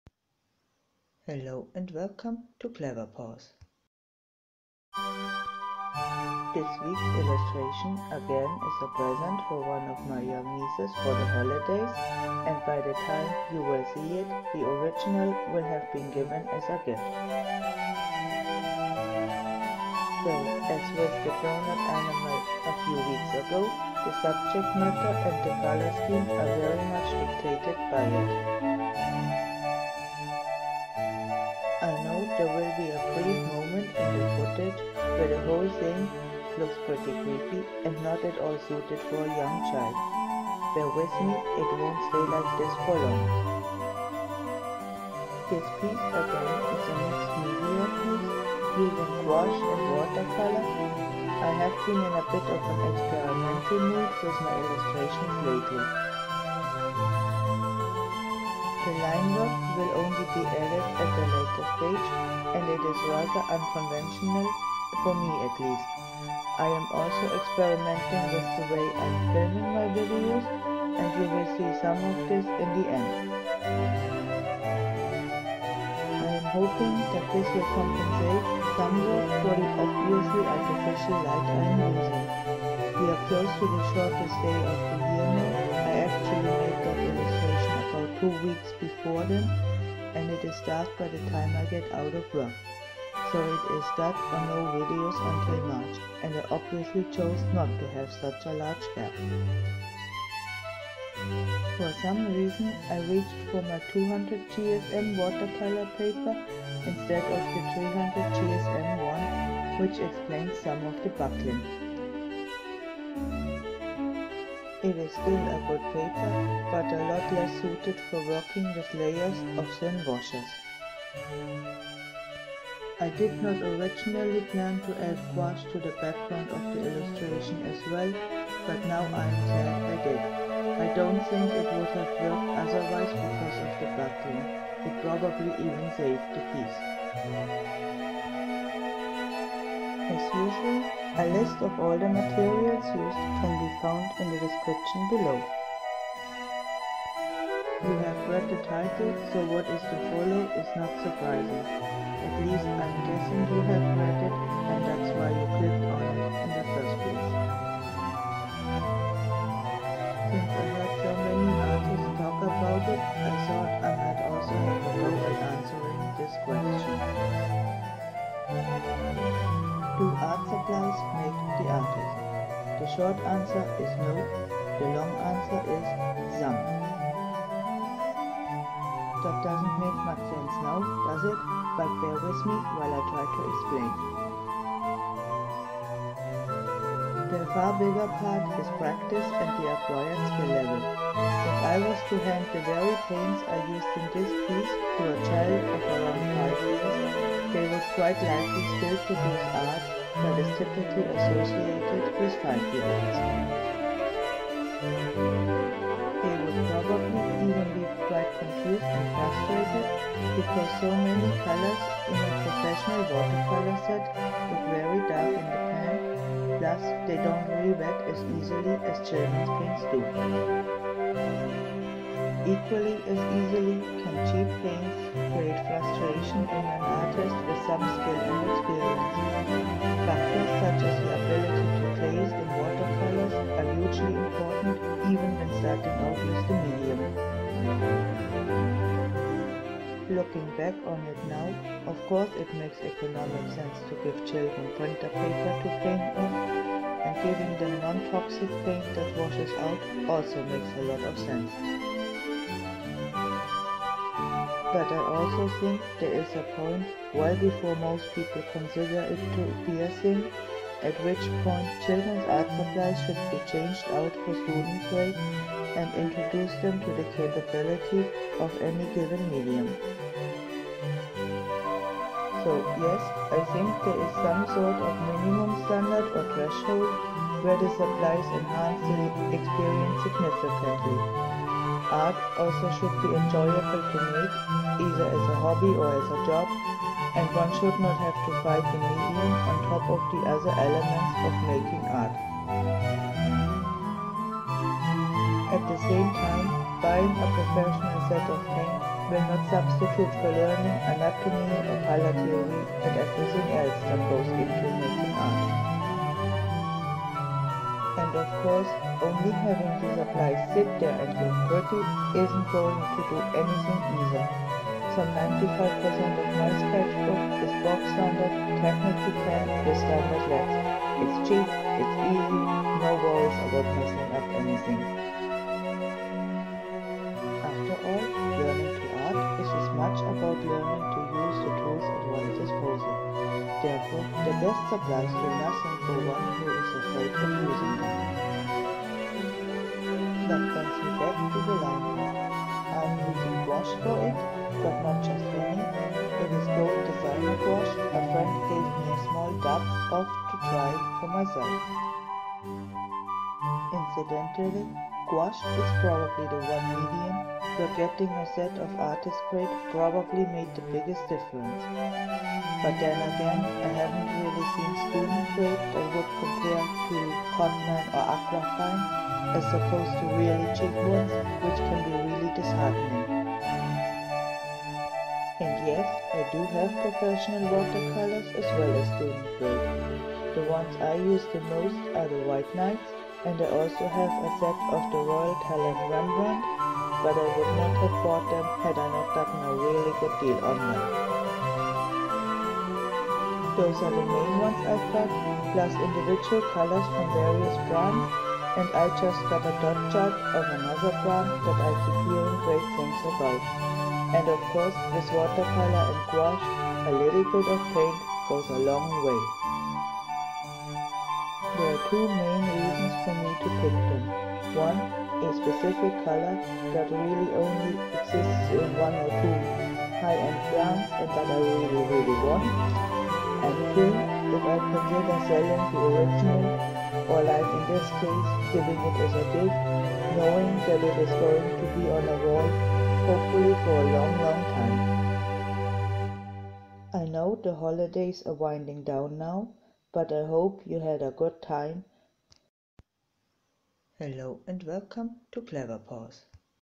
Child's Teddy – Mixed Media Speedpaint – Do artsupplies make the artist?
Happy 2019.
This is the first video of the year here on my channel, and we are starting off with some more experimenting in my arts. If you stick to the end, I am not only trying some new ways of filming, but also in the lineart.

Materials:
200gms Ami Watercolour paper
Royal Talon Rembrandt Watercolorus
Windsor and Newton and Daler Rowney Designer Gouache